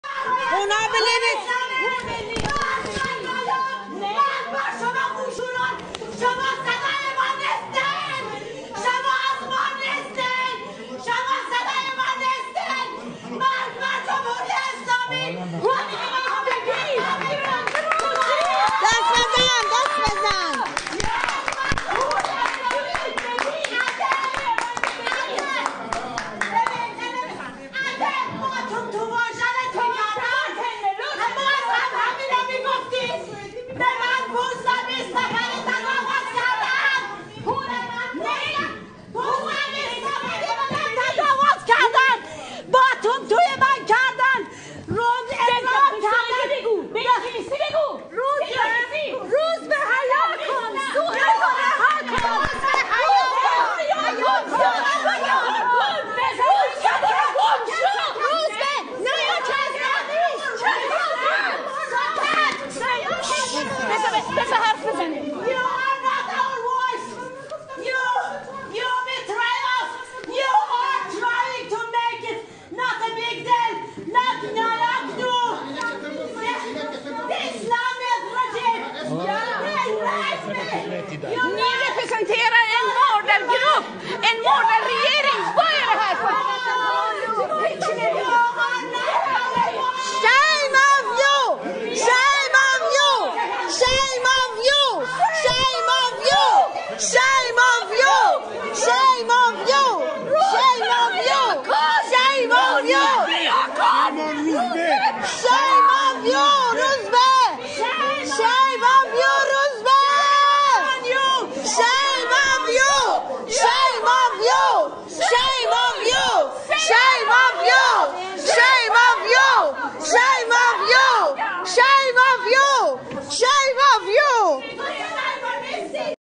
حضور و اعتراض جمعی از مخالفین جمهوری اسلامی در جلسه سخنرانی روزبه پارسی - در استکهلم سوئد
حضور و اعتراض جمعی از مخالفین جمهوری اسلامی در جلسه سخنرانی روزبه پارسی از عوامل رژیم برای عادی سازی چهره جنایتکار نظام در ساختمان موزه مدیترانه در استکهلم سوئد

این جلسه  قرار بود در اکتبر امسال برگزار شود اما با شروع و اوج اعتراضات بهم خورد. سپس با تغییر زمون به سه شنبه ۶ دسامبر  موکول شد.

روزبه پارسی  برادر تریتا پارسی که هردو  از سران نایاک هستند و روزبه این نقش را در سوئد و اروپا سالهاست دارد از قبل برای همین  اکتبر گذشته جلسه ای توجیهی در مورد جنایات رژیم ایران و عادی سازی ان برای سوئدی ها داشت که با اوج گرفتن اعتراضات دعوت شدگان و شرکت کنندگان از ان صرفنظر کردند.